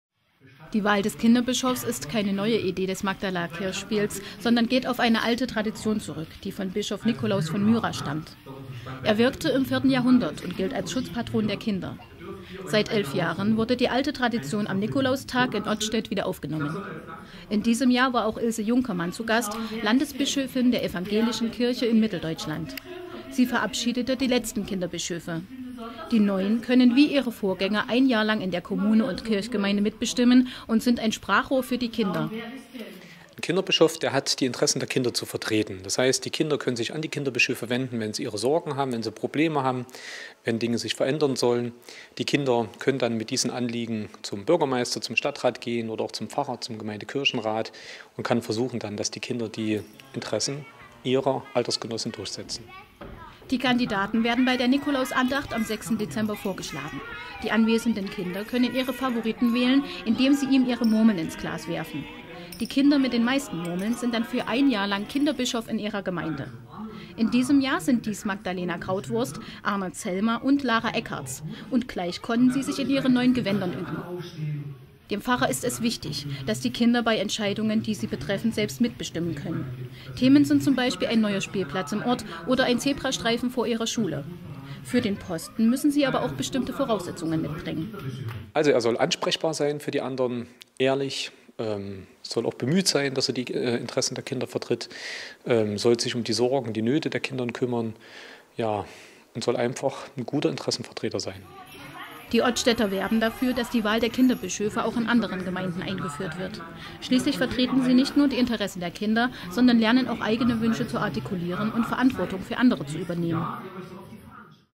Kinderbischöfe gewählt: In Ottstedt können auch Kinder in der Kirche mitbestimmen
In der evangelisch-lutherischen Kirchgemeinde Magdala / Bucha dürfen auch Kinder mitbestimmen. Seit 11 Jahren werden in der Kirche in Ottstedt am Nikolaustag Kinderbischöfe gewählt. Diese sollen die Interessen der Kinder vertreten und ihr Sprachrohr sein.